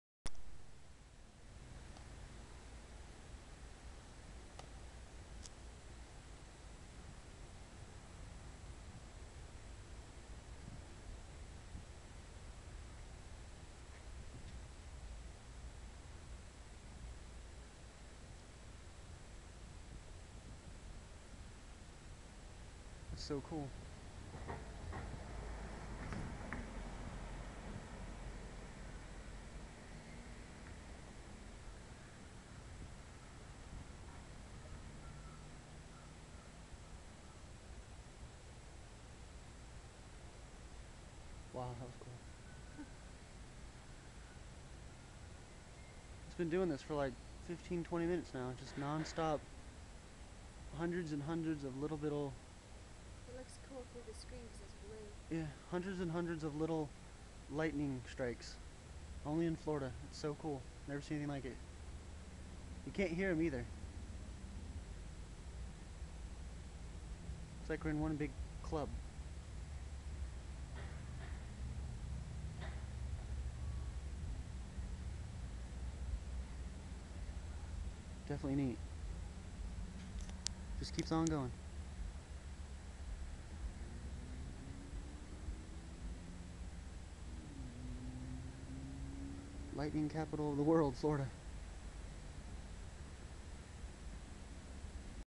Florida Lightning Strikes
I was outside the garage and hundreds of these small lightning strikes were going off.  It was the coolest thing.